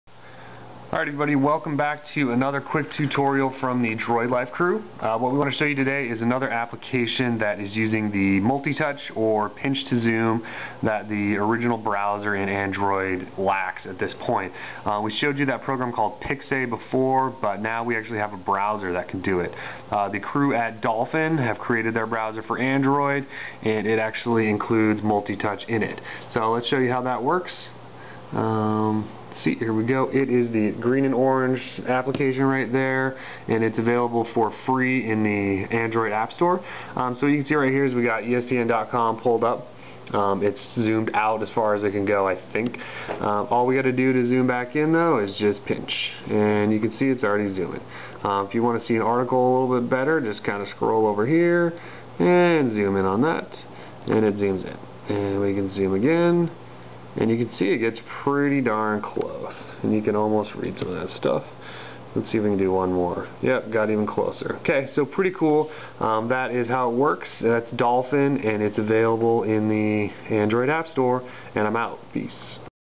Motorola Droid - Dolphin Browser Includes Multi-Touch
motorola droid tips and tricks! new dolphin browser is free in the android market and includes pinch-to-zoom or multi-touch!